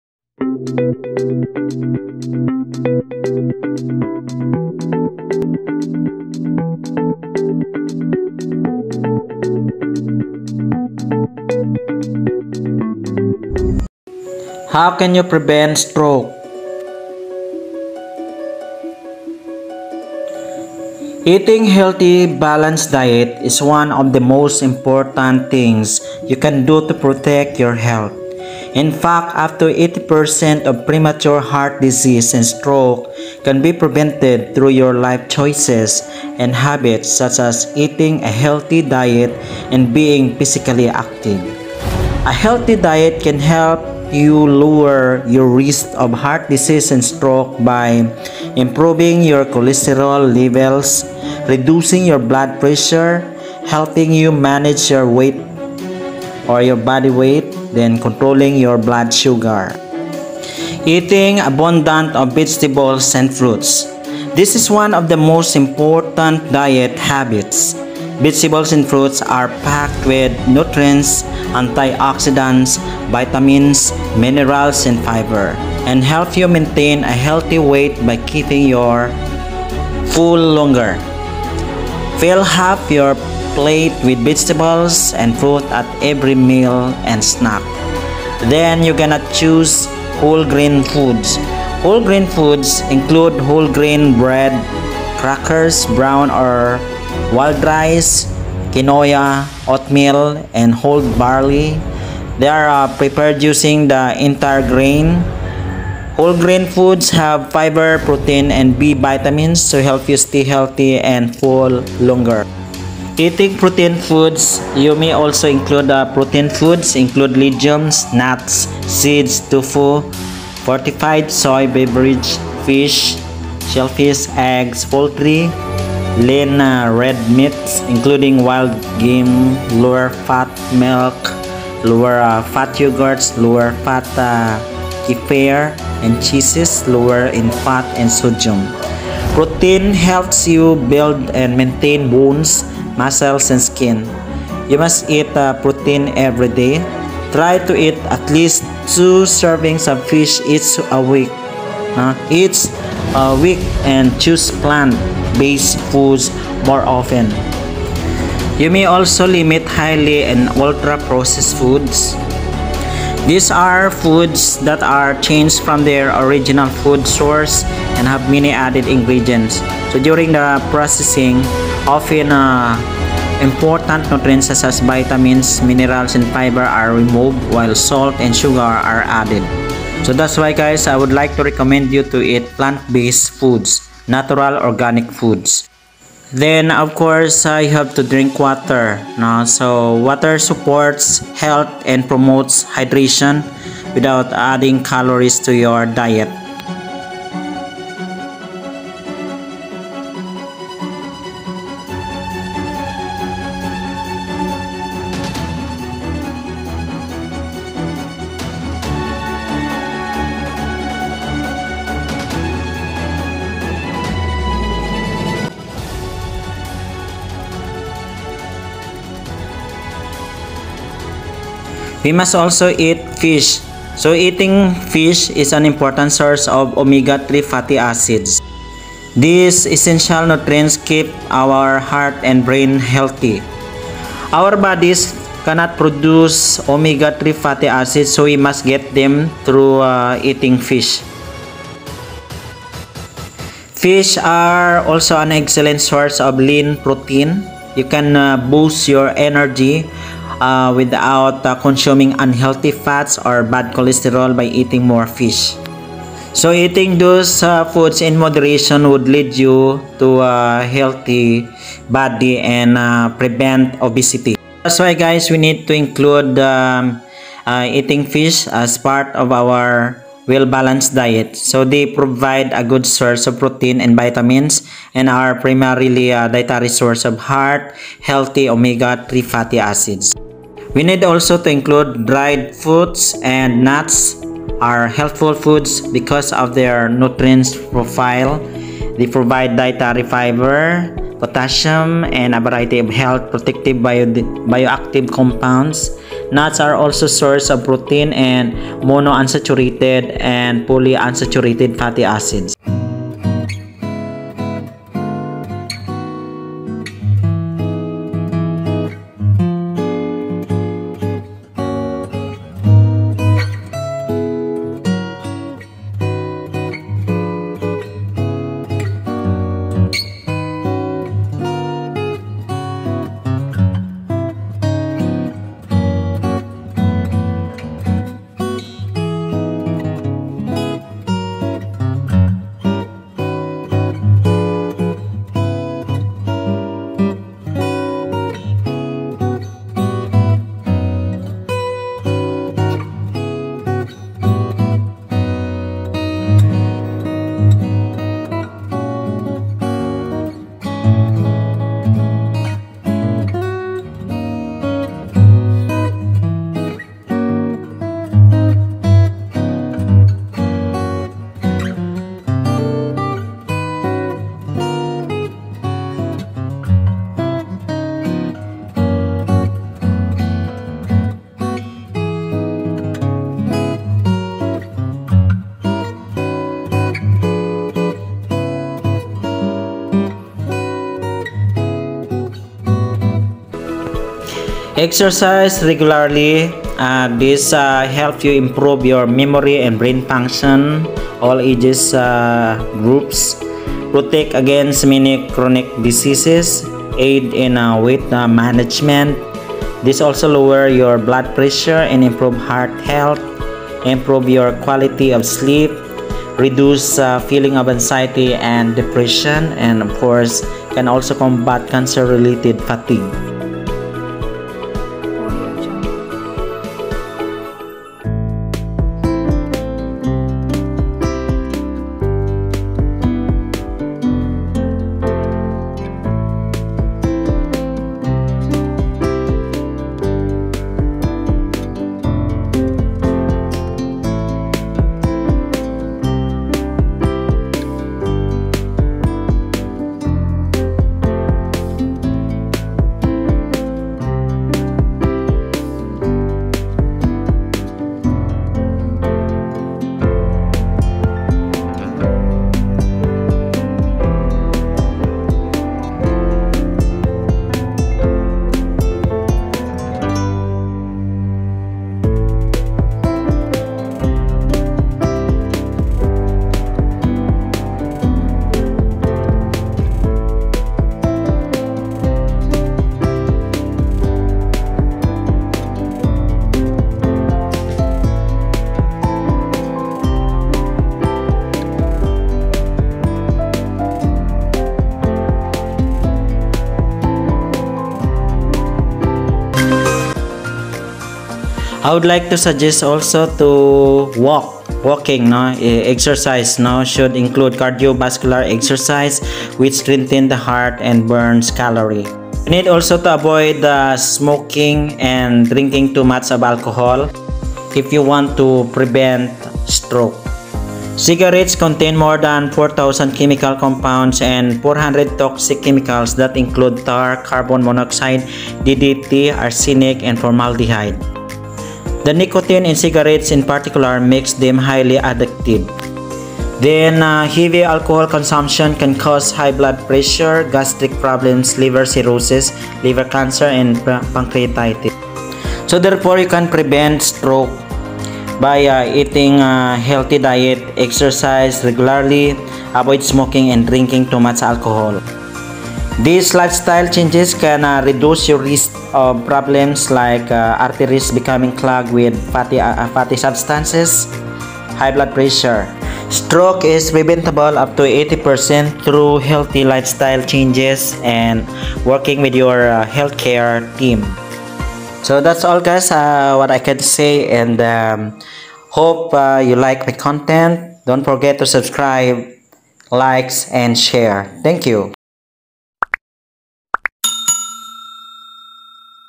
HOW CAN YOU PREVENT STROKE I HEALTH TIPS
Stroke can be prevented through the following:
1.Eat a Healthy Diet
2. Exercise Regularly
3.Avoid smoking and drinking too much
  alcohol.
These lifestyle changes can reduce your risk of problems like: arteries becoming clogged with fatty substances(atherosclerosis)high blood pressure.